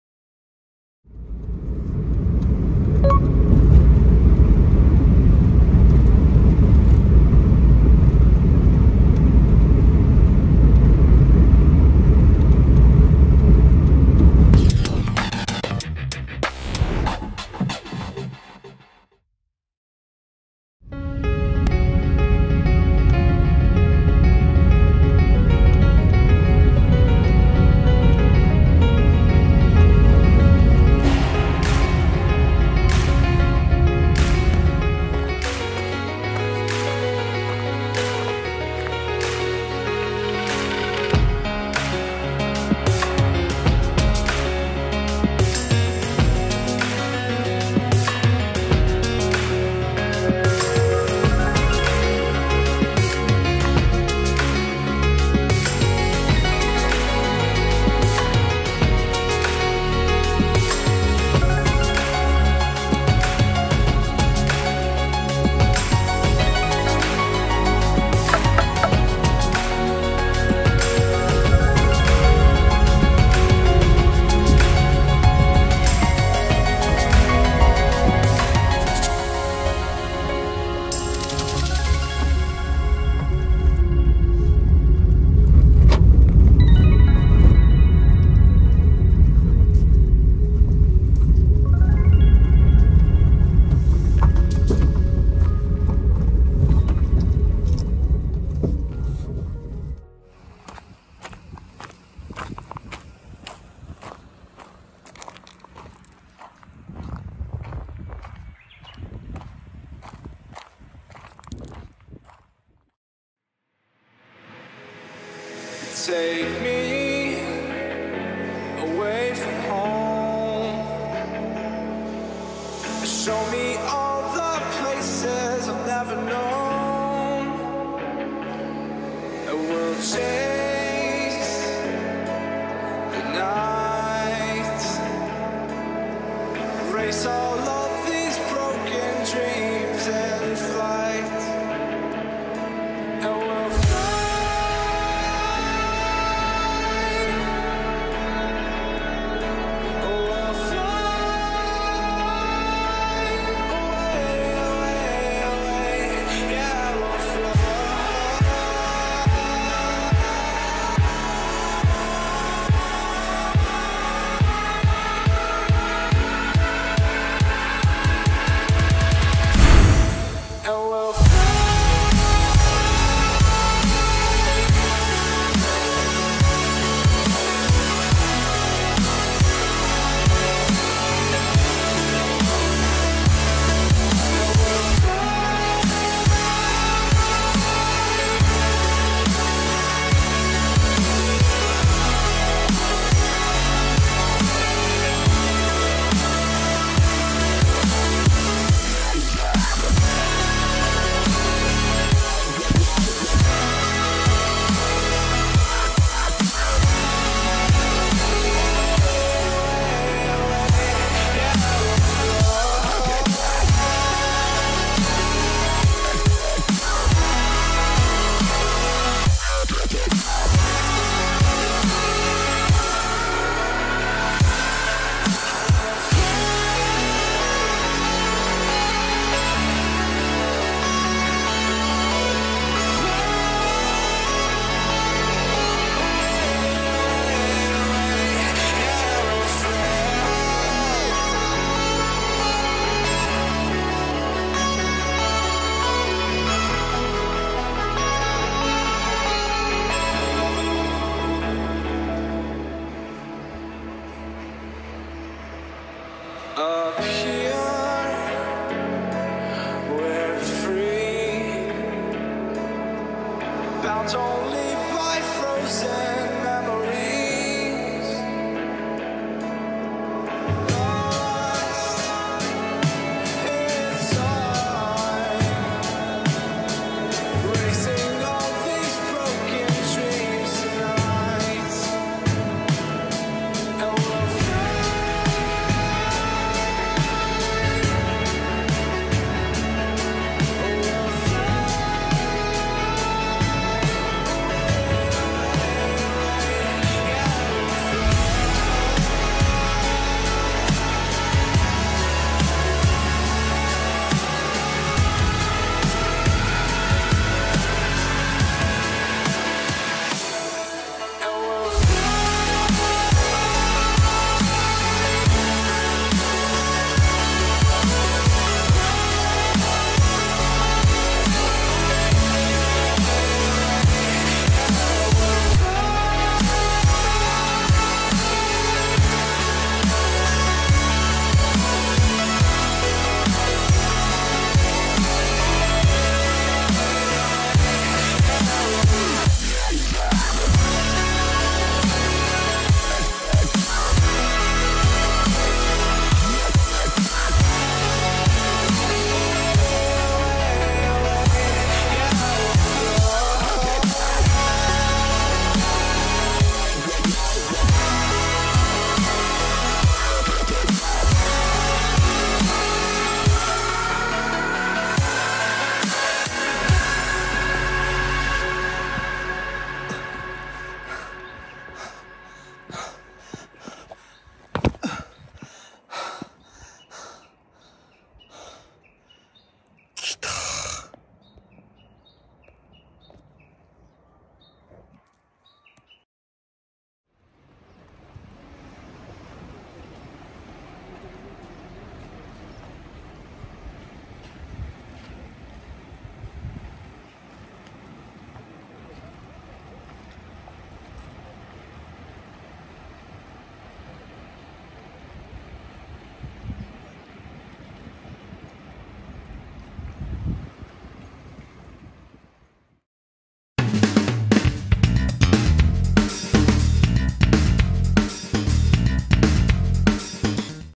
【ドローン空撮】三重県熊野市の秘境！木津呂集落を空撮 / 秘境探索 / 地方創生・地域活性化
今回は知る人ぞ知る三重県熊野市の秘境！木津呂集落を撮影して来ました🎥こちらは龍穴と呼ばれるパワースポットとしても有名だそうです。
幸い天気にも恵まれて気持ちよくドライブと撮影を楽しんで来ました😄木津呂集落の周りを囲むように流れる北山川は瀞峡(どろきょう)と呼ばれ、紅葉の季節は船に乗って紅葉を眺められる人気のアクティビティが楽しめるそうです🍁